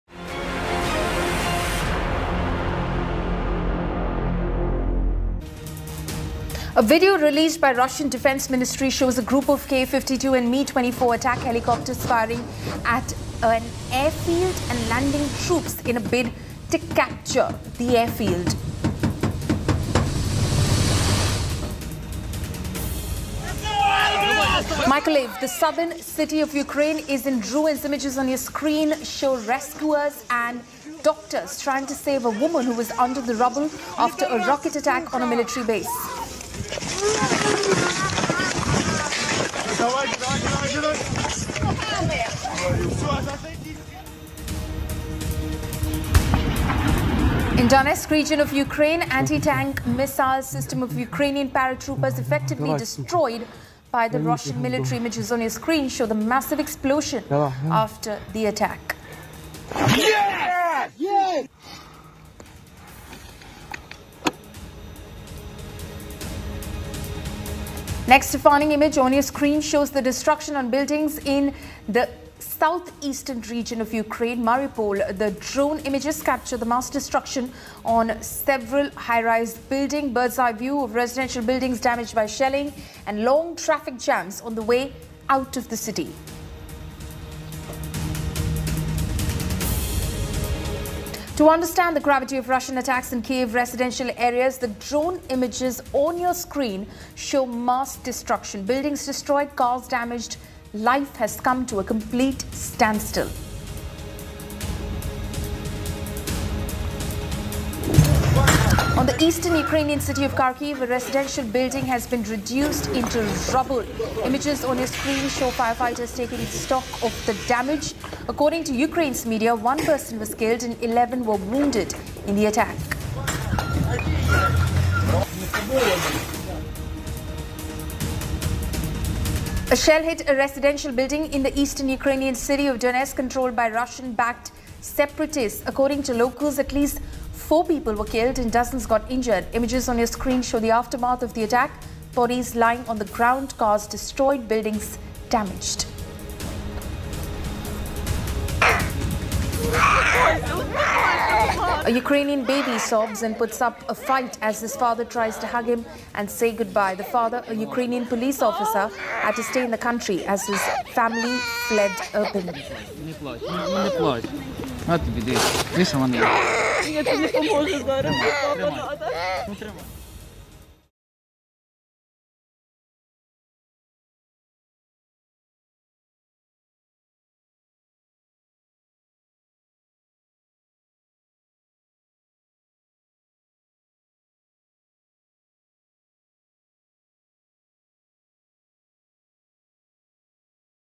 Russia-Ukraine Stand-off | Take A Look At The Top Most Defining Images Of Russia's Ukraine Invasion
India Today brings the topmost defining images of Russia's Ukraine invasion.